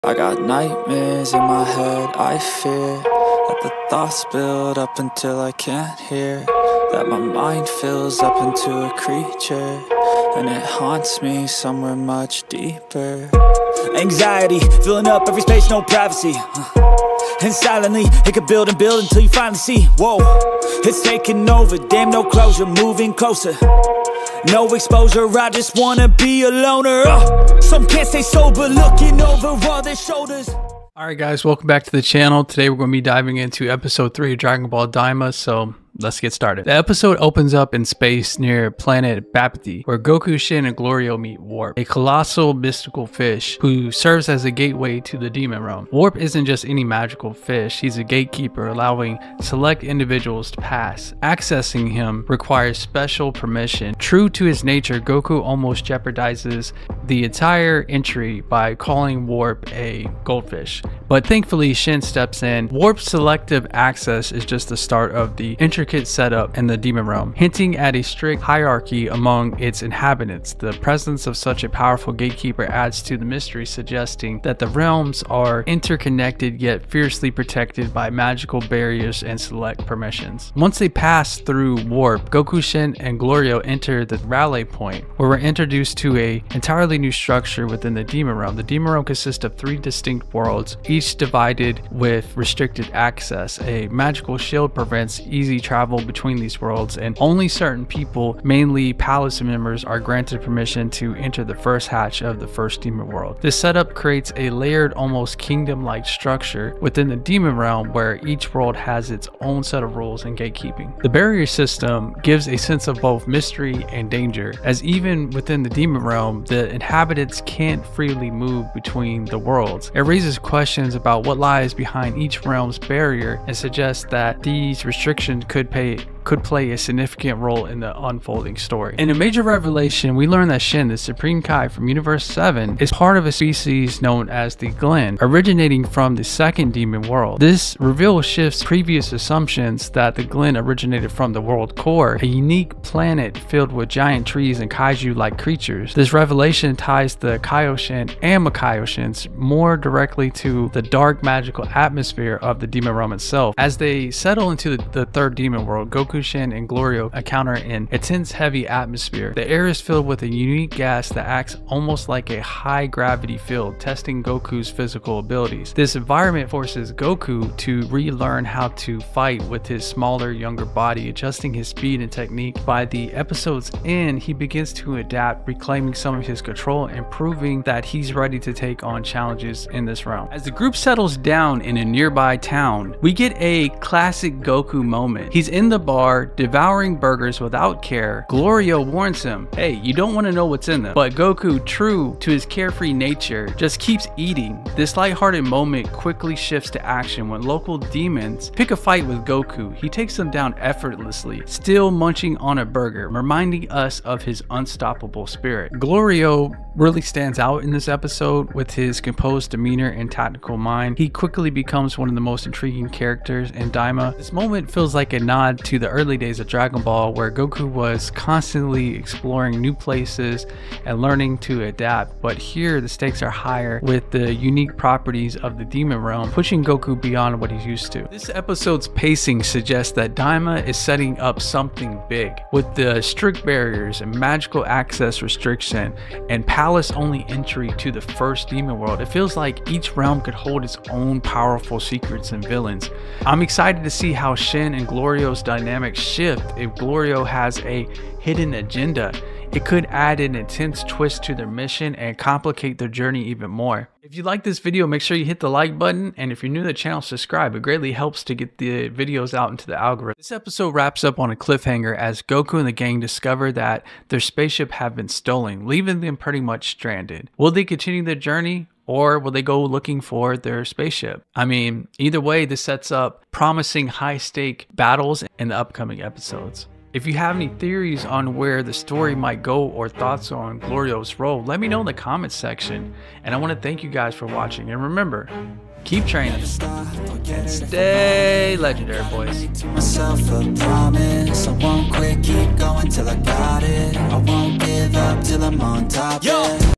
This is Incredible 1000% it will Change Everything
In Dragon Ball Daima Episode 3, Goku, Shin, and the mysterious Glorio venture deep into the Demon Realm, unlocking secrets that could change everything! They encounter Warp, a colossal mystical fish who holds the key to accessing hidden realms—but only for a select few. Goku must adapt quickly as he battles against the realm’s crushing gravity and new foes. With Glorio’s mission to stop the powerful King Gomah, dark secrets about the three divided Demon Worlds and Shin’s true origins begin to surface. Could this be Goku’s most dangerous challenge yet? Don’t miss out on all the explosive details and shocking reveals in this latest Daima breakdown!